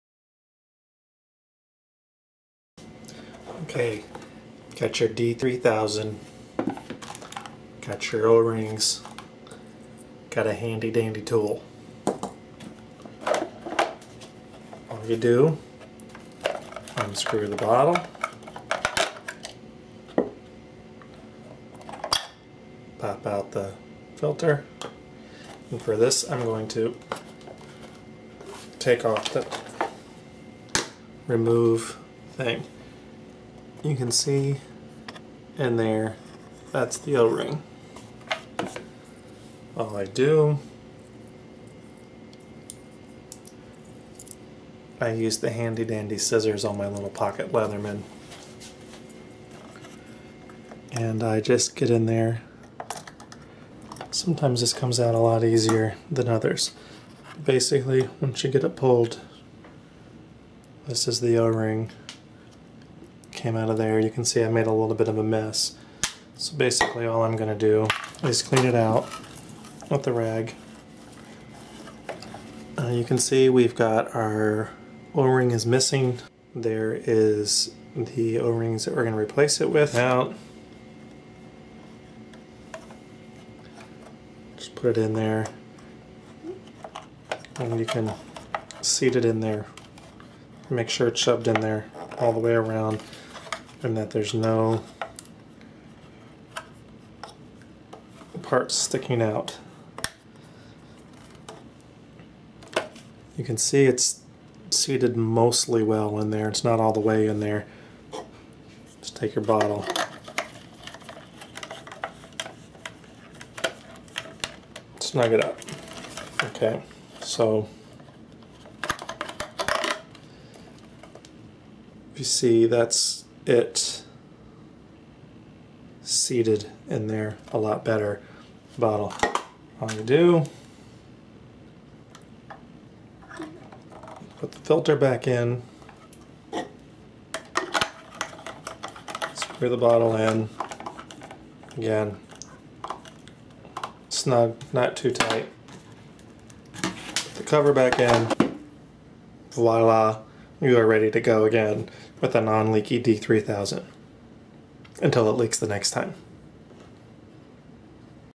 D3000 O Ring Change
How to change the O-Ring on the D3000 by an amateur.